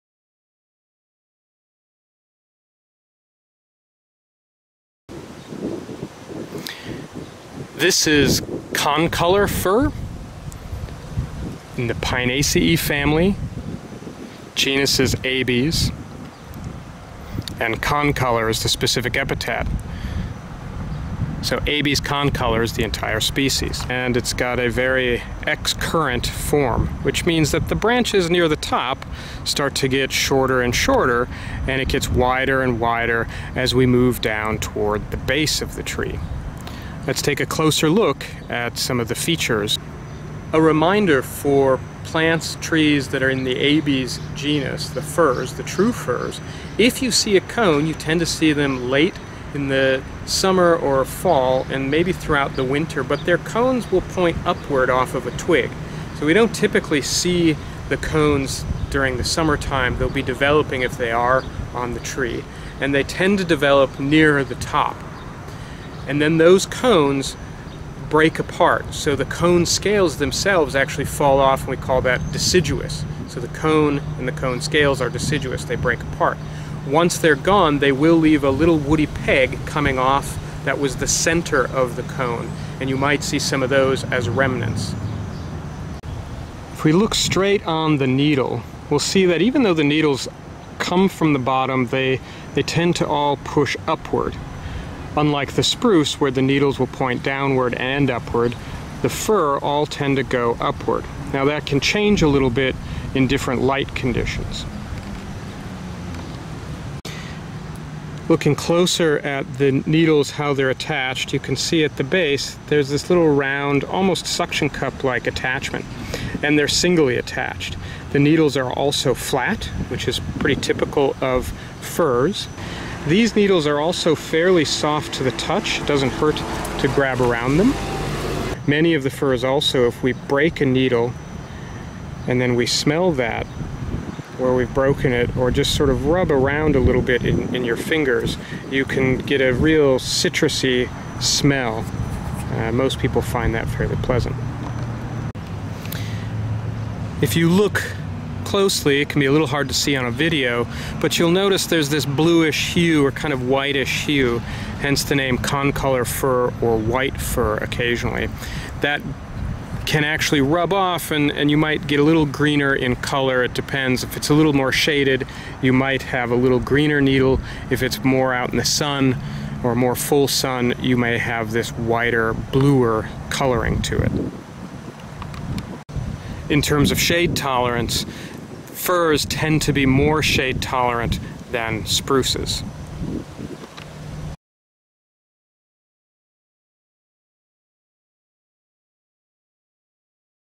Identifying concolor (white) fir
Identification of concolor (white) fir - Abies concolor

Video created by to for the course NRES 201 - Dendrology to supplement class lectures and field walks.